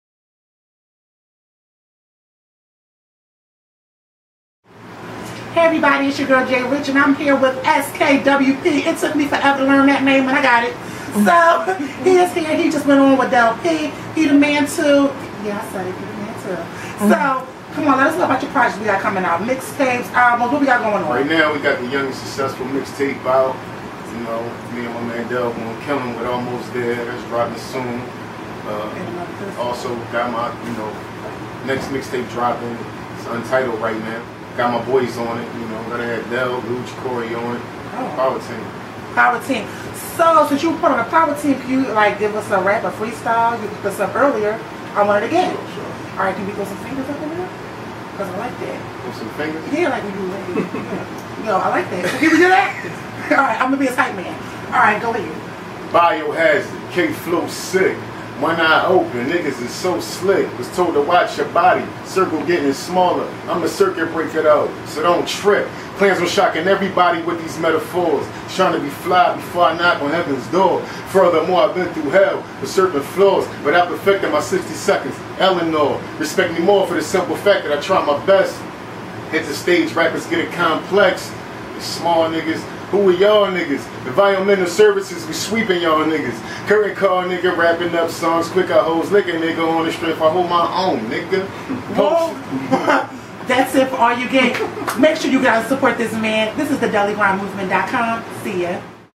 Ski-Wp with The Daily Grind
Ski-Wp is very lyrically inclined and has a style that is unmatched. We had so much fun talking to him and getting to know more about his journey. To listen to the interview please check out